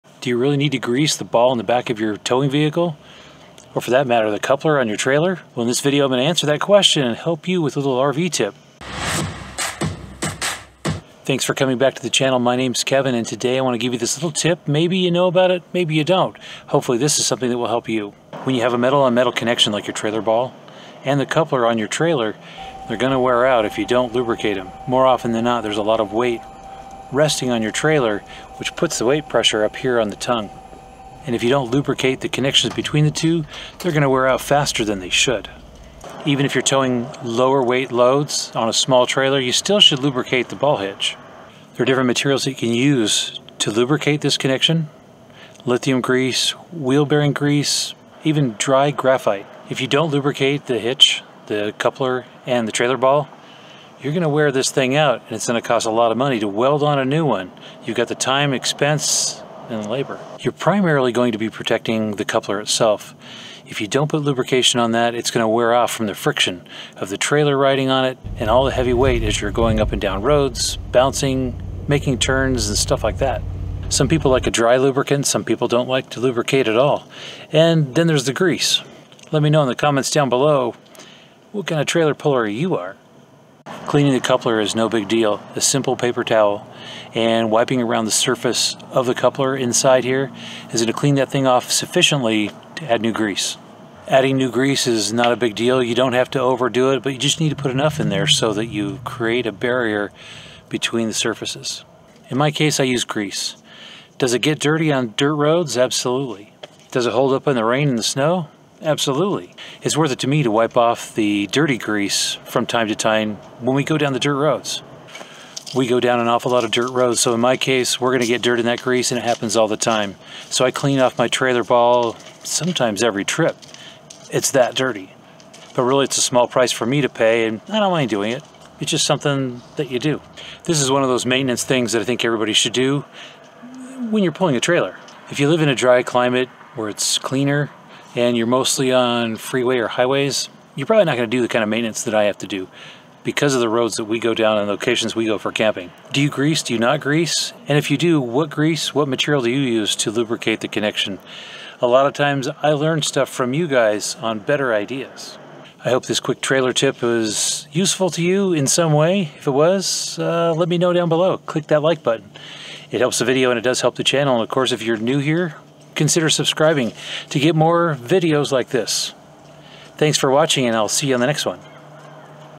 Trailer Hitch Maintenance | RVAddict
Trailer Hitch Maintenance is on the top of our checklist for each trip.
Inspecting and lubing the tow ball and trailer coupler connection on each trip is really important.
This is what I am sharing today.

00:00 Begin
01:00 Why lubricate
01:23 Cleaning the coupler